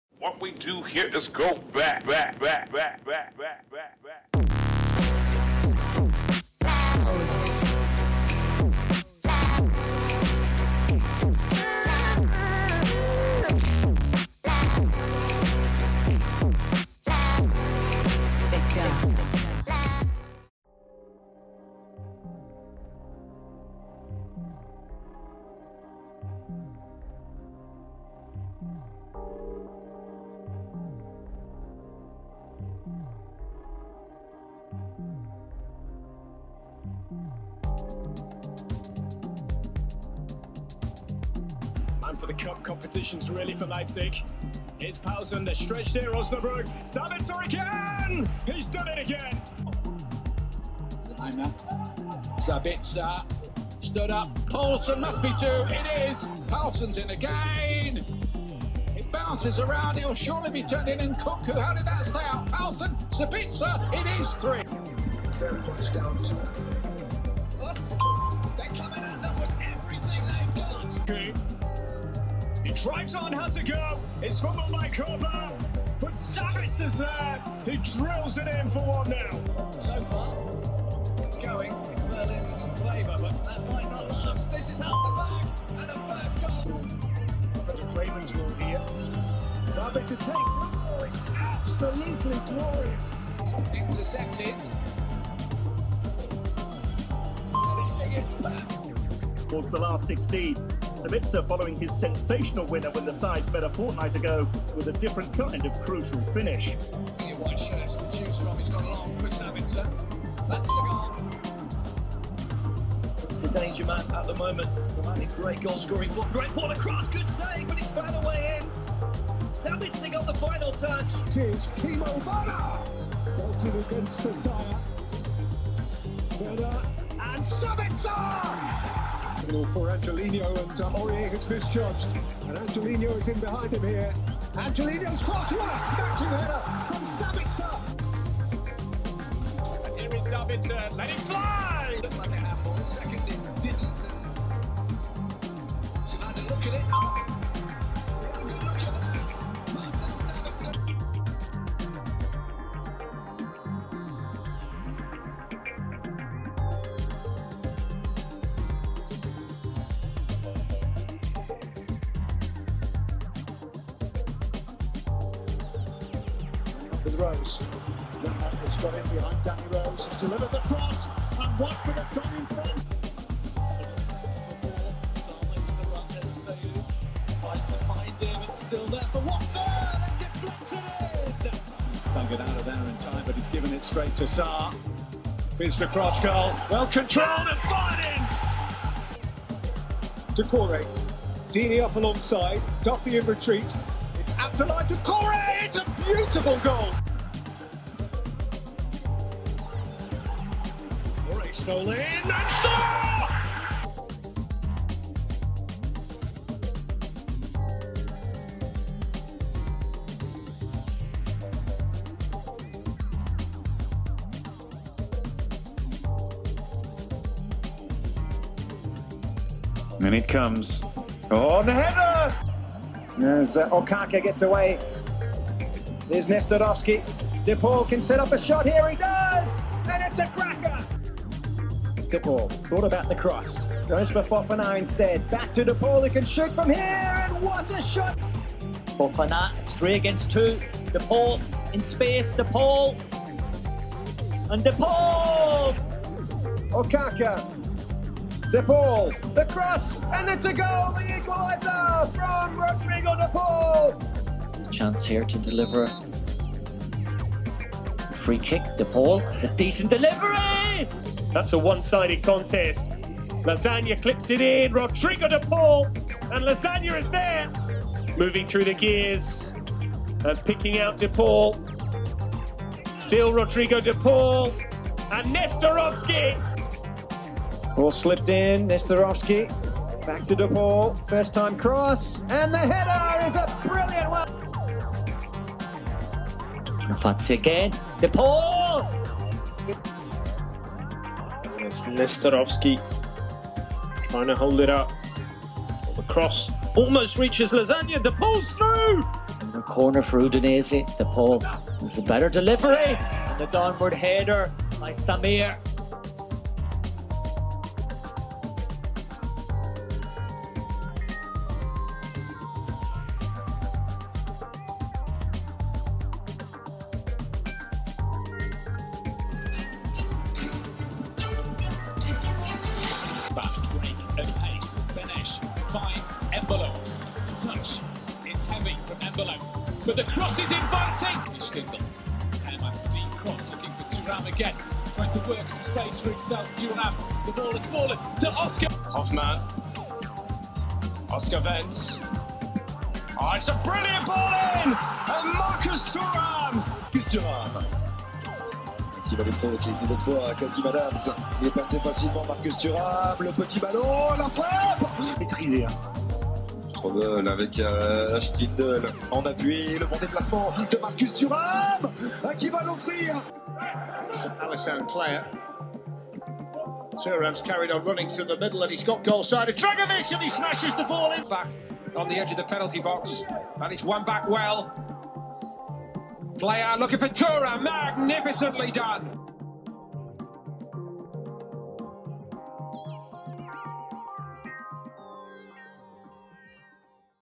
4 UNDER THE RADAR PLAYERS ARSENAL MUST SIGN
Out of Marcel Sabitzer, Abdoulaye Doucore, Rodrigo De Paul and Marcus Thuram, who would you most like Arsenal to sign?
Any players I have missed?